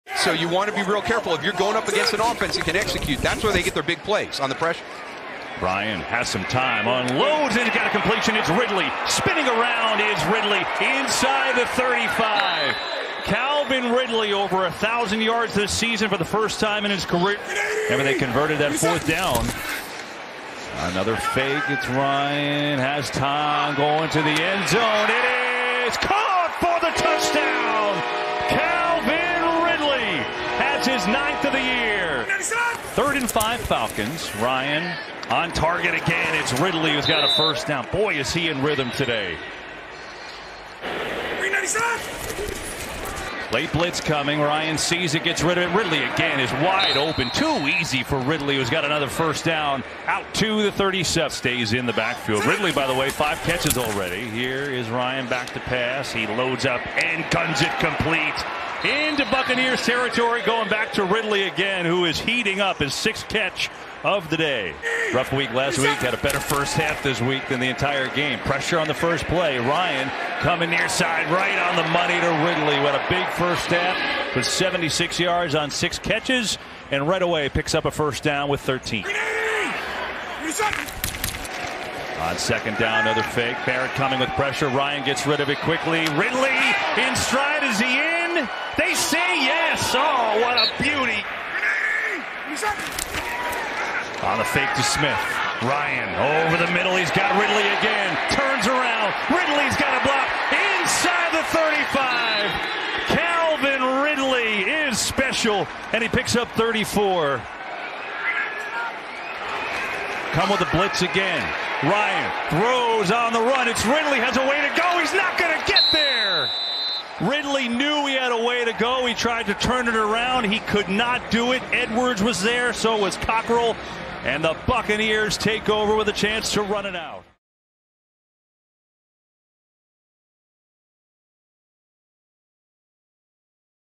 Calvin Ridley Goes OFF w/ 163 Yds & 1 TD | NFL 2020 Highlights
Calvin Ridley gave another incredible performance to make up for sidelined Julio Jones, catching 10 receptions for 163 yards and a touchdown. The Tampa Bay Buccaneers take on the Atlanta Falcons during Week 15 of the 2020 NFL season.